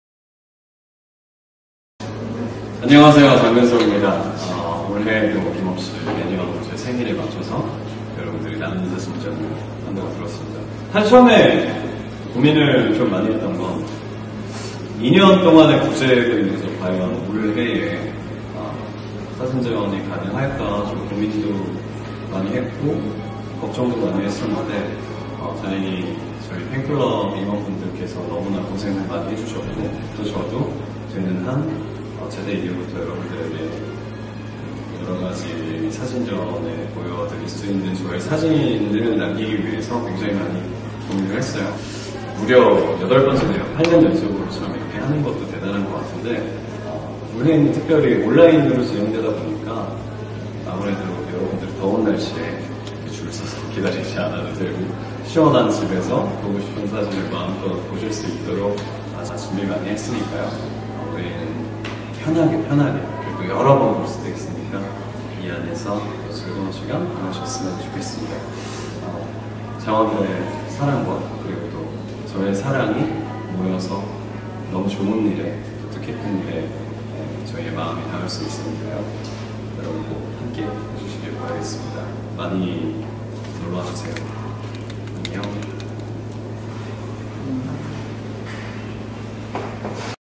birthday crij
here  is the video filmed by Jang Keun Suk, whose birthday is coming on Aug. 4.  On his birthday, his official FANCLUB called Cri J will collect all the money donated from his fans all over the world and then send the money to people in need. This is the 8th year for this sharing event. With his love from all his fans, the good deeds will absolutely go on in the future.